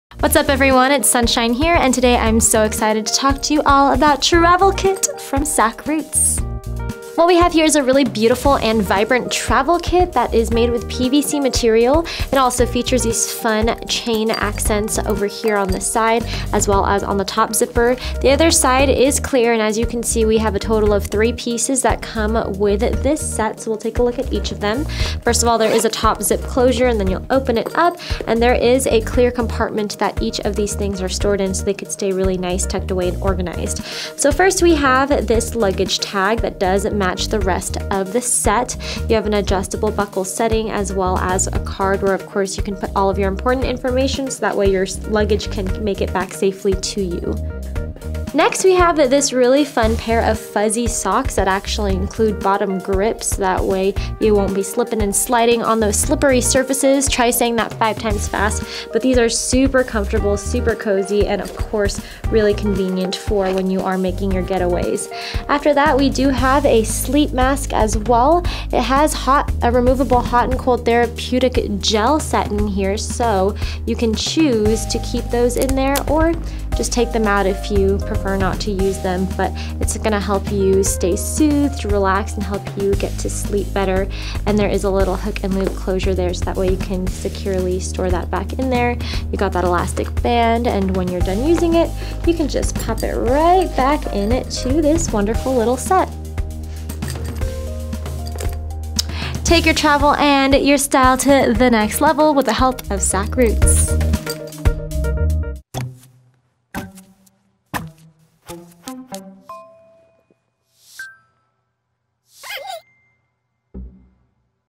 Sakroots Travel Kit SKU:8853066
It's time to getaway, make sure it's a cozy commute with the perfect Travel Kit.
Set includes 3 pieces.
Set includes:
Clear pouch featuring a tassel and critter keychain
Luggage tag
Fuzzy socks with bottom grips
Eye mask with removable hot/cold therapeutic gel
Made of PVC.
Zipper closure.
Imported.
Measurements:

    Bottom Width: 10 in
    Middle Width: 10 in
    Top Width: 10 in
    Depth: 2 in
    Height: 8 1⁄2 in
    Weight: 5.4 oz